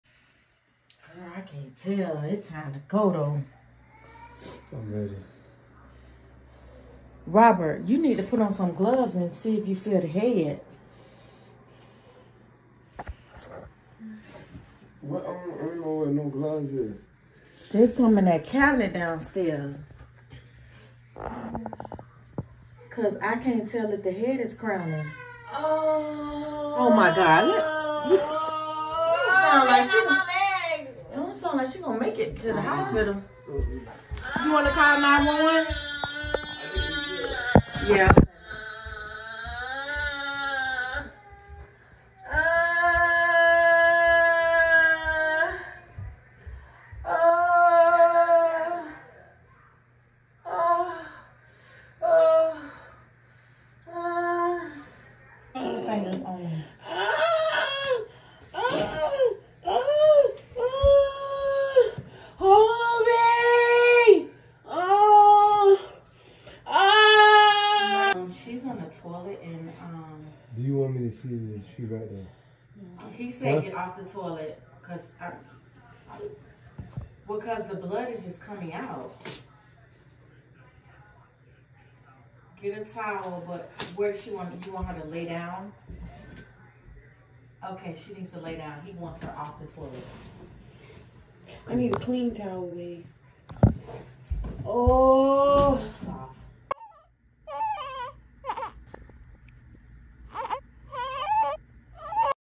We Had Another Baby
We welcome our new addition to the Ortiz family !!! Welcome baby girl ! 💕🤱🫶🏾